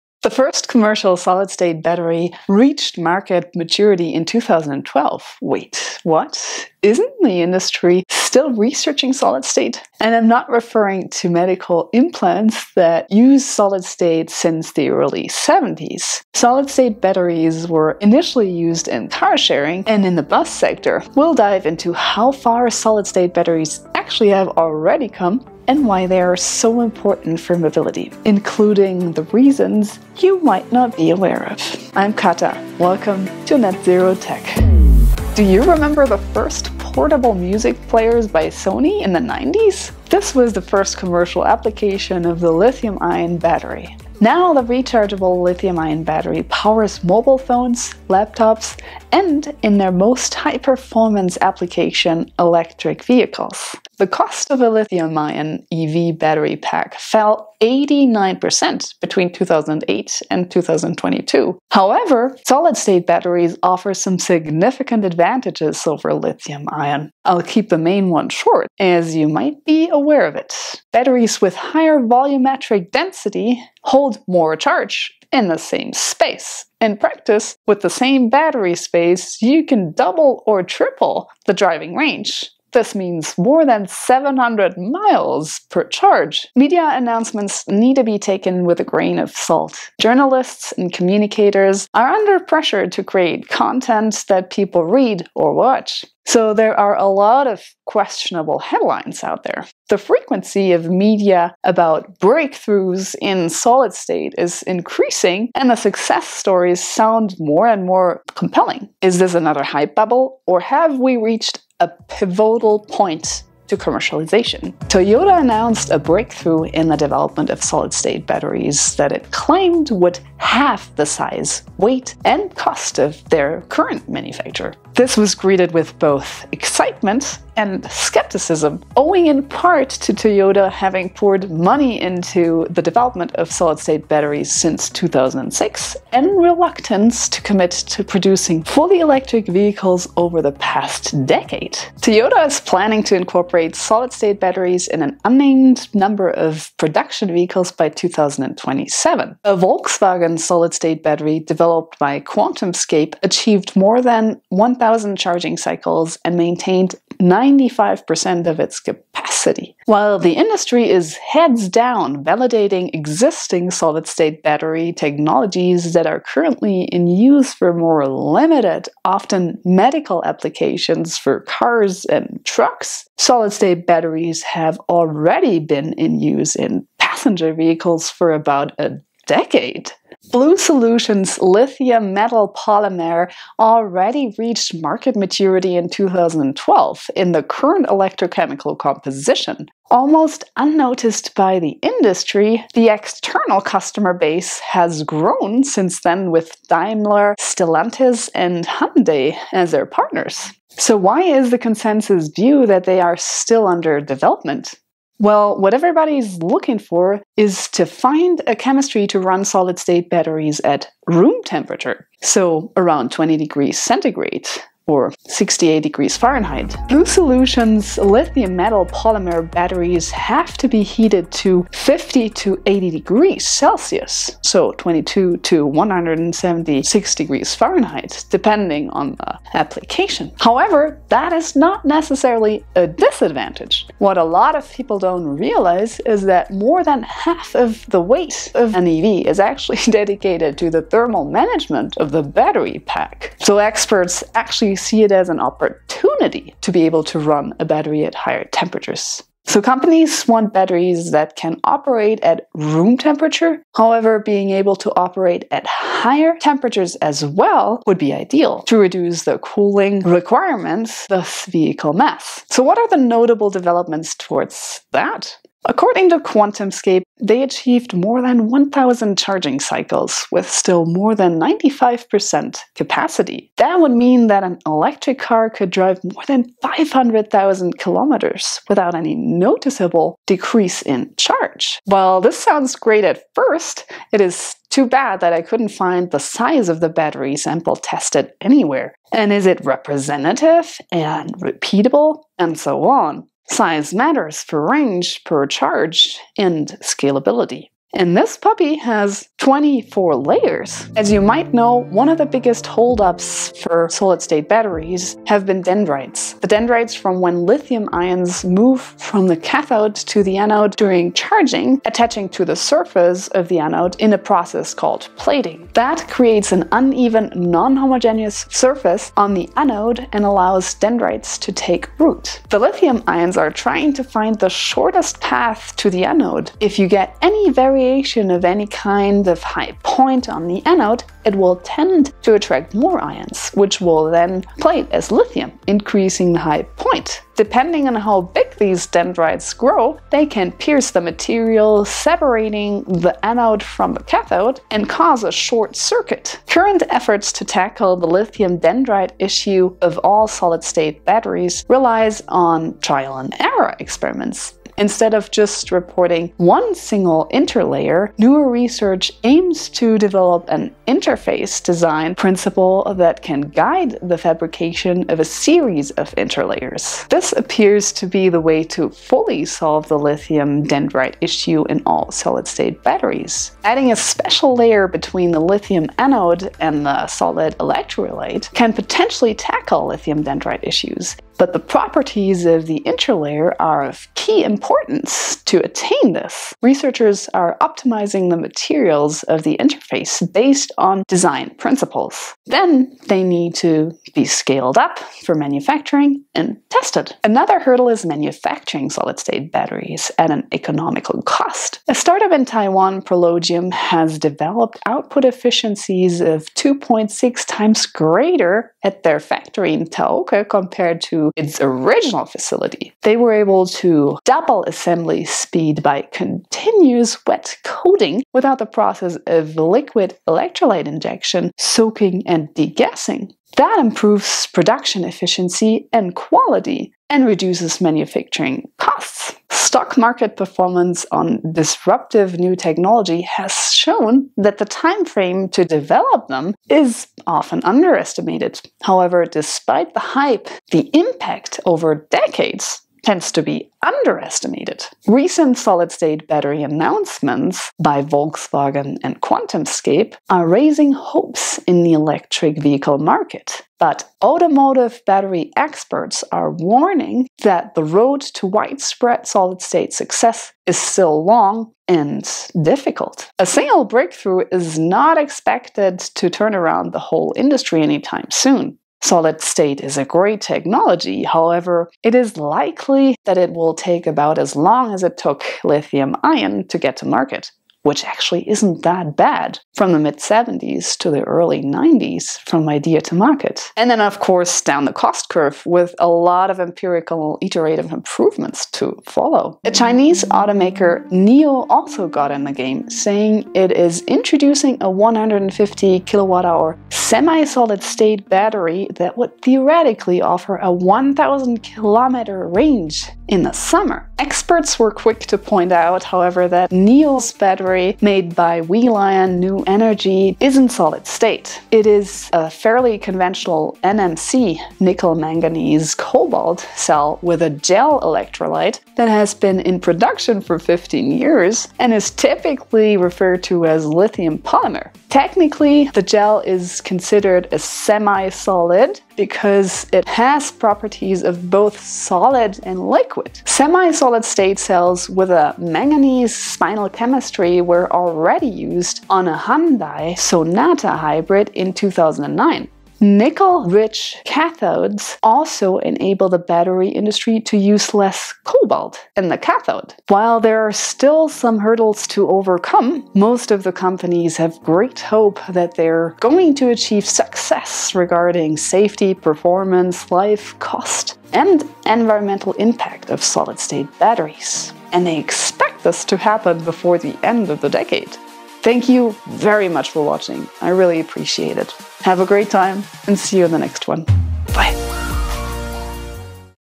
Why Solid-State Batteries are here (and why that matters)
The first commercial solid-state battery reached market maturity in 2012. Wait, what? Isn’t the industry still researching solid state? While medical devices use solid state since the early '70s, solid-state batteries were initially used in car-sharing and in the bus sector. We’ll dive into how far solid-state batteries actually have already come and why they are so important for mobility including a few reasons you might not even be aware of...

0:00 - Intro
0:34 - Lithium Ion Batteries - The Predecessor
1:02 - Why Solid State Batteries matter - Energy Density, Range, etc.
1:54 - Will Toyota win the Race? - The latest "breakthrough"
3:00 - Is BlueSolutions a Decade ahead? - The operating Temperature Range
3:53 - Battery Temperature Management and Vehicle Mass
4:25 - What's the "sample size" of Volkswagen and QuantumScape's SSB? - 1,000+ cycles and 500,000 km range at 95% capacity
5:00 - What's the Holdup? - Dendrites and Solution Approaches
6:37 - There is more... - Manufacturing
7:10 - Hype or Solid Future? - The Stockmarket, the Media and Experts
8:09 - What about the Chinese Automaker NIO? - NMC with a Gel Electrolyte, Semi-Solid Battery in the Hyundai Sonata
09:09 - Conclusion

🔔  Your subscription to the channel goes a long way.